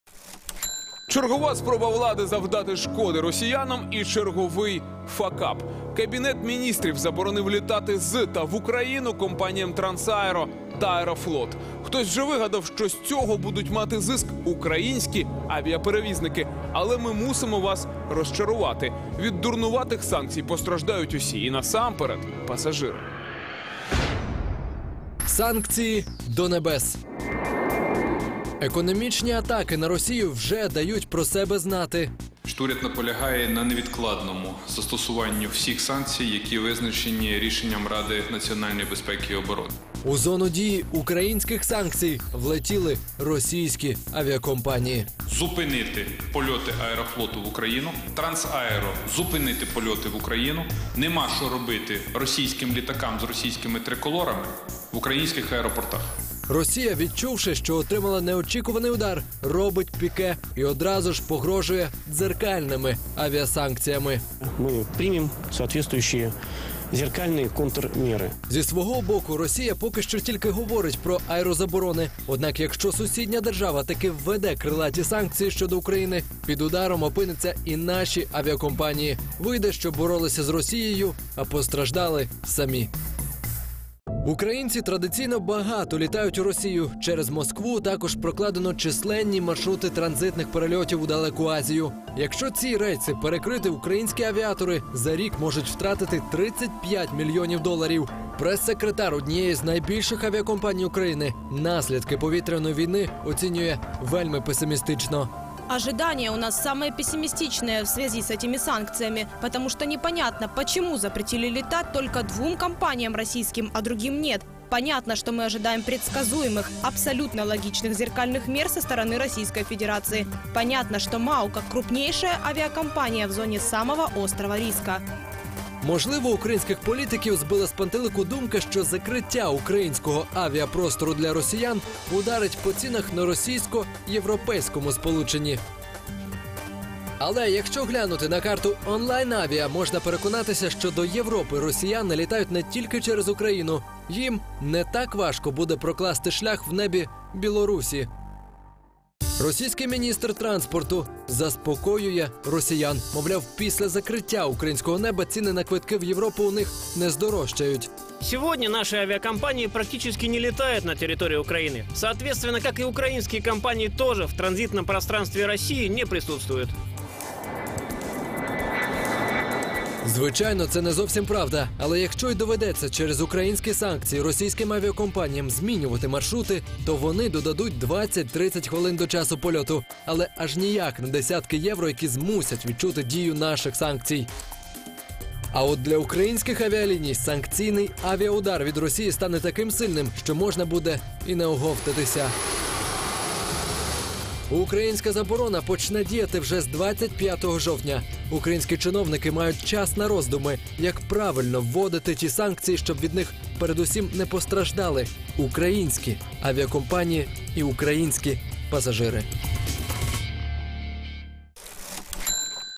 Гроші. Санкційні авіарозборки
Чим для нас обернеться закриття українського неба для російських авіакомпаній. Заборона на українське небо. Чи допоможуть Україні санкції на російські авіакомпанії та хто з українських авіаперевізників може потрапити у чорний список Росії у відповідь.